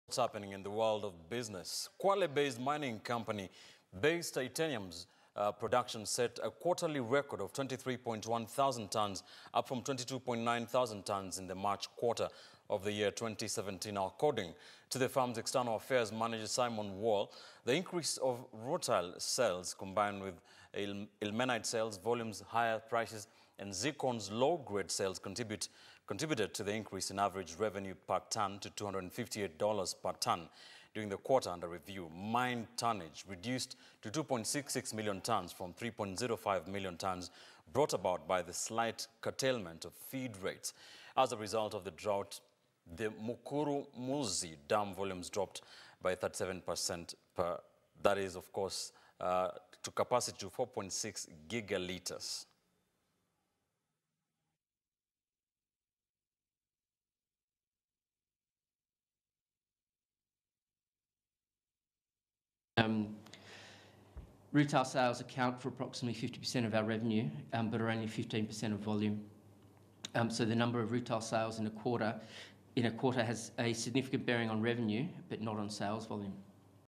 Titanium production has grown up comp Kwale in the March quater of 2017
Kwale based mining company base titanium’s rutile production set a quarterly record of 23.1 thousand tonnes up from 22.9 thousand tonnes in the march quarter of 2017.according to the firms external affairs manager Simon wall, the increase of rutile sales combined with limonite  sales volumes, higher prices and zircon’s low grade sales contributed to the increase in average revenue per tonne to 258 dollars per tonne.  During the quarter under review, mined tonnage reduced to 2.66 million tonnes from 3.05 million tones brought about by the slight curtailment of feed rates. As a result of the drought, the mukurumudzi dam volumes dropped by 37% of capacity to 4.6 gigalittres. Though sustainable water measures have been implemented, the mining company is seeking regulatory approval to increase prefield extraction  to help mitigate future risks.